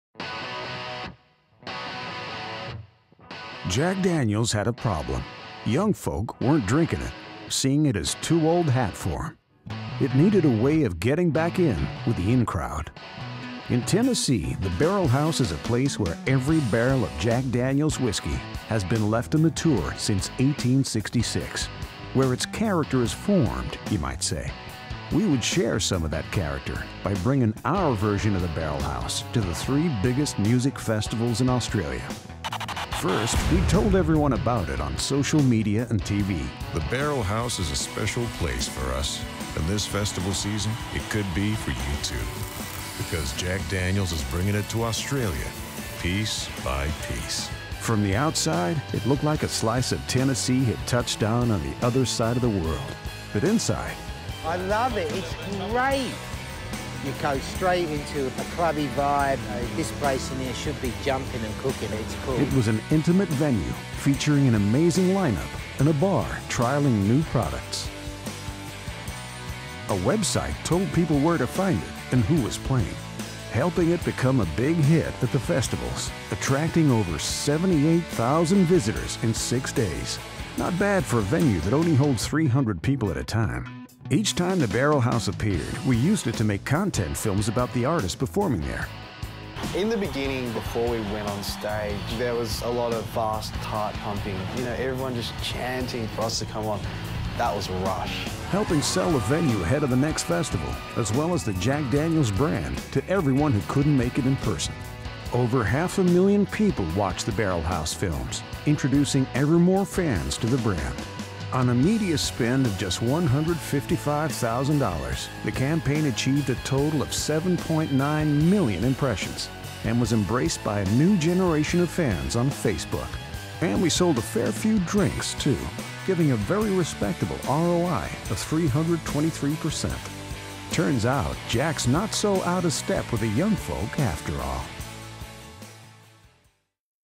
Jack Daniel's Barrelhouse
From the outside, The Barrelhouse was a beautiful reproduction of Jack Daniel’s original, 163 year old distilling house in Lynchburg Tennessee. But inside, it was an intimate live music venue and bar that toured the 3 biggest music festivals in Australia.

Each time it appeared we used it to create new content films about the artists performing there, then posted the content online to sell the venue ahead of the next festival, as well as the Jack Daniel’s brand to everyone who couldn’t make it in person.